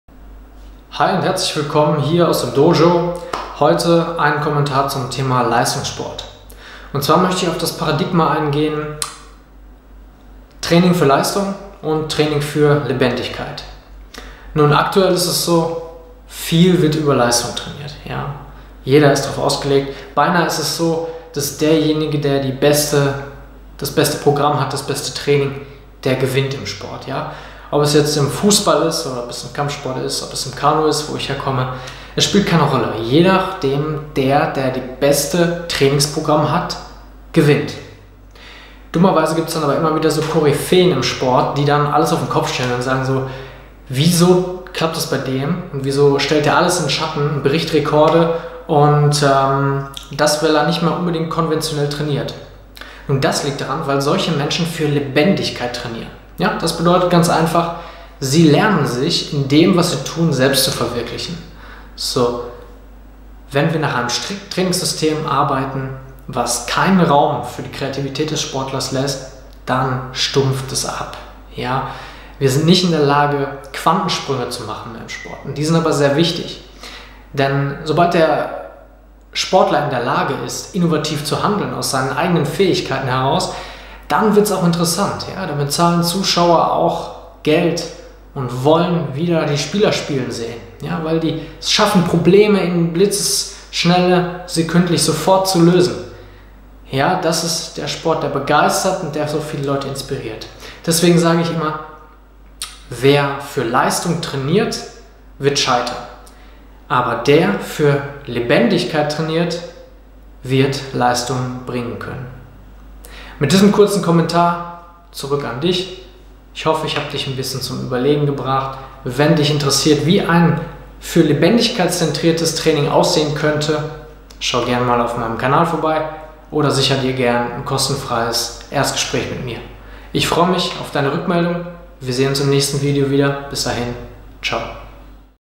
Tipps für Spitzensportler - Lebendigkeit vs Leistung
Ist Leistung und Lebendigkeit nicht das Selbe? Wenn du Fragen hast, melde dich einfach!
Mehr Infos im persönlichen Gespräch ; )
Lars Krieger -
Transformationscoaching
"Ich führe Menschen zu erneuerter Stärke & Bedeutung"